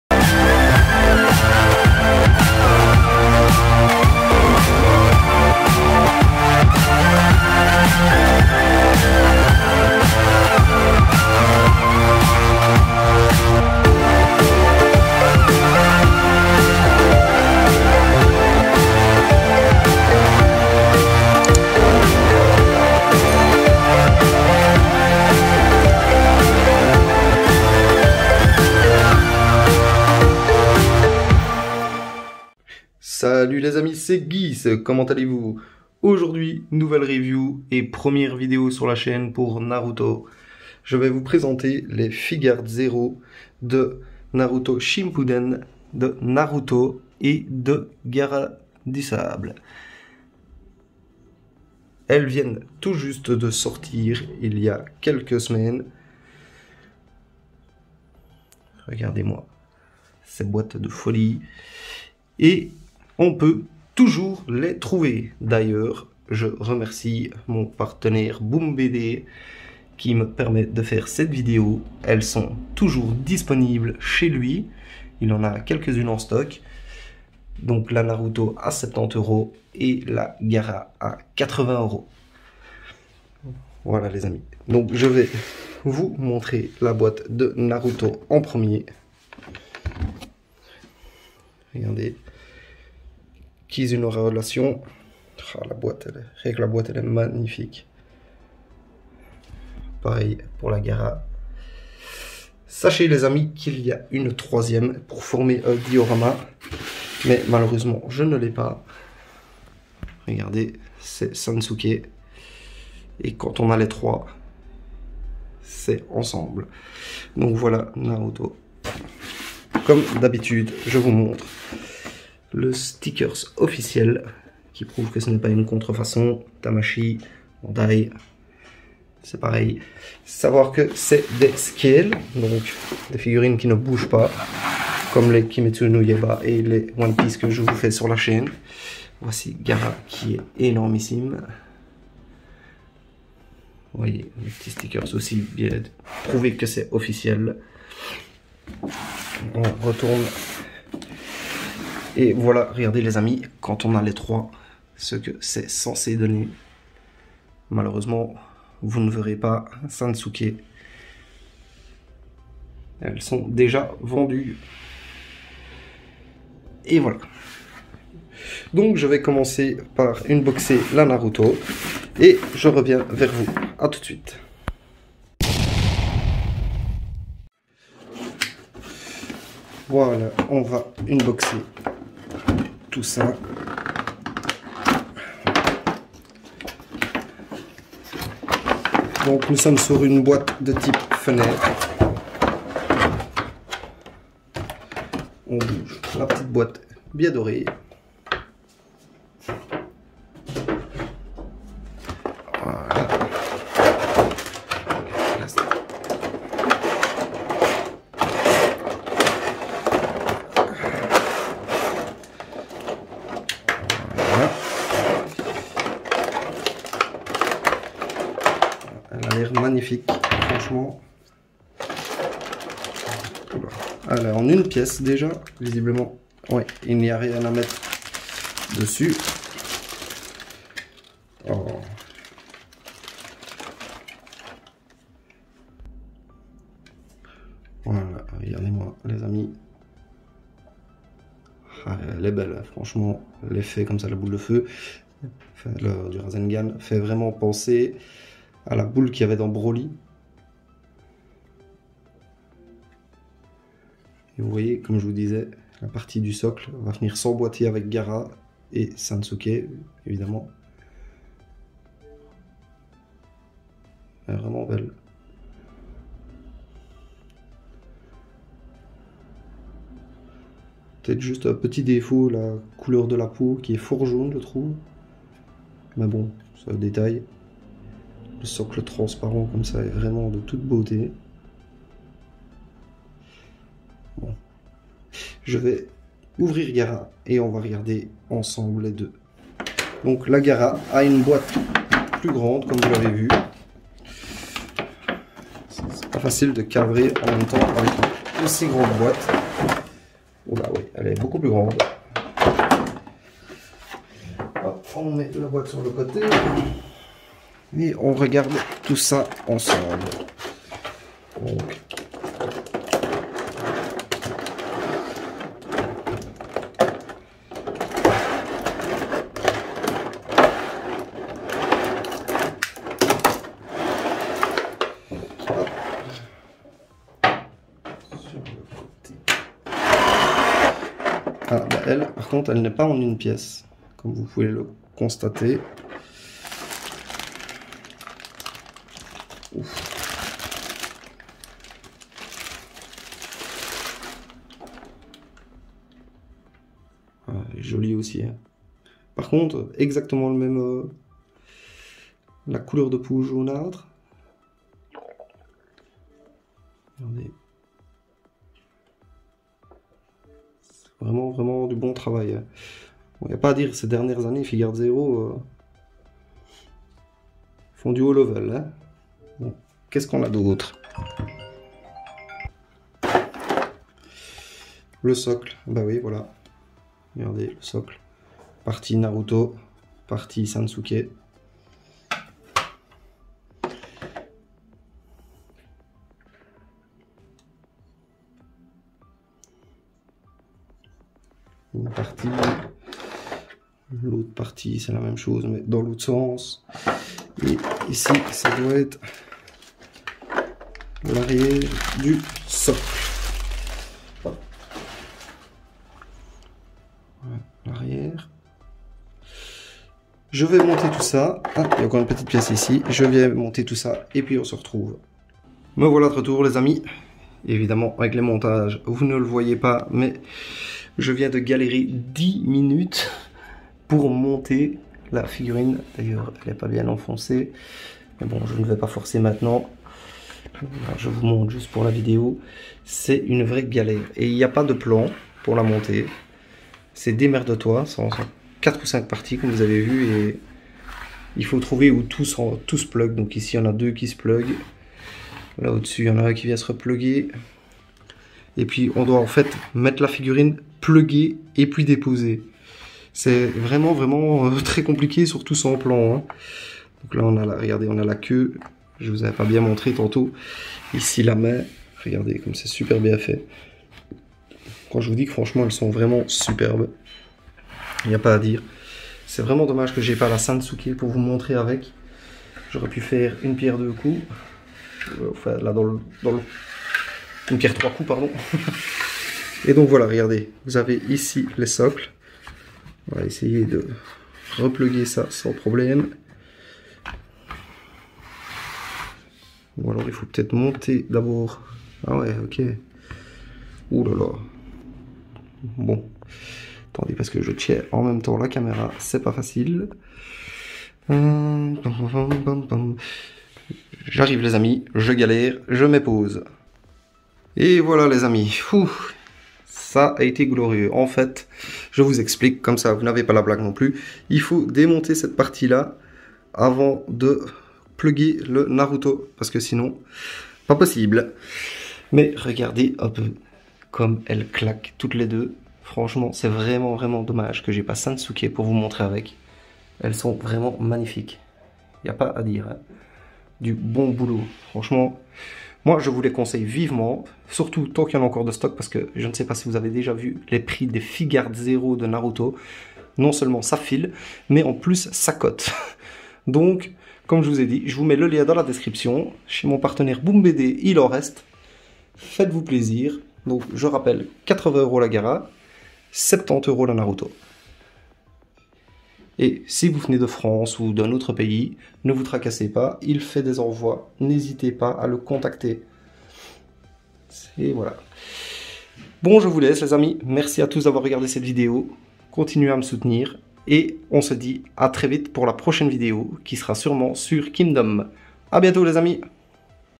Figuarts Zero Naruto Uzumaki/ Gaara Kizuna Relation Unboxing
Salut les amis, première vidéo sur la chaîne sur la licence naruto shippuden, je vous présente non pas une mais deux figuarts zero MAGNIFIQUES de naruto et gaara , malheureusement je n'ai pas encore sasuke pour vous montrer avec le diorama fini ... mais ce n'est que partie remise :)

Description :

Figurine : Naruto
Taille : 19 cm
Matière : PVC
Licence/Manga : Naruto Shippuden
Série : Figuarts Zero
Marque : Tamashii Nations
Fabricant : Bandai Spirits

Figurine : Gaara
Taille : 17 cm
Matière : PVC
Licence/Manga : Naruto Shippuden
Série : Figuarts Zero
Marque : Tamashii Nations
Fabricant : Bandai Spirits